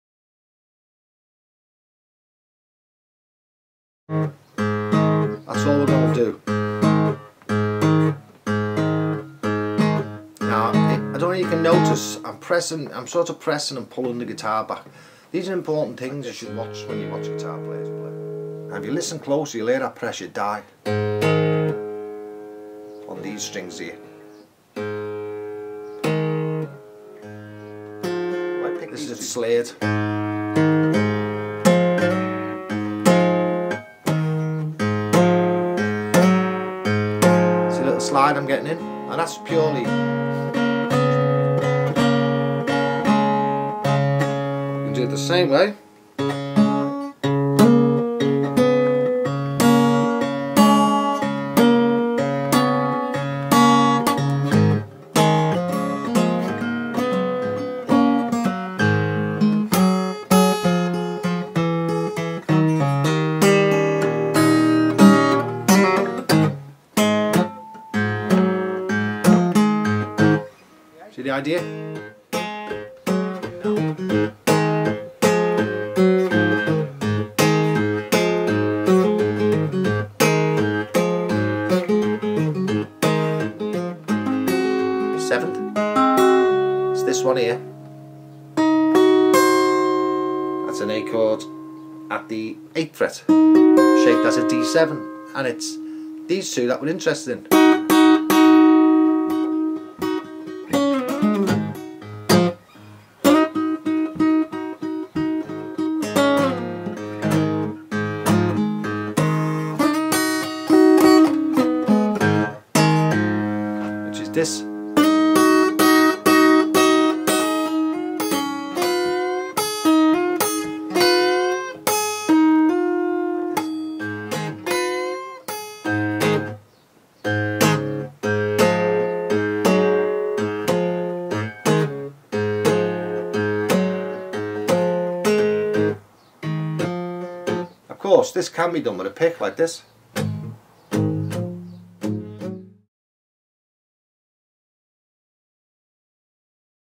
Taster from Acoustic Toolbox part 3. Classic rock shuffle
This is a master class in the techniques you can put on a classic rock shuffle. A must have for anyone wanting to master this technique. The full lesson is 1 Hour long.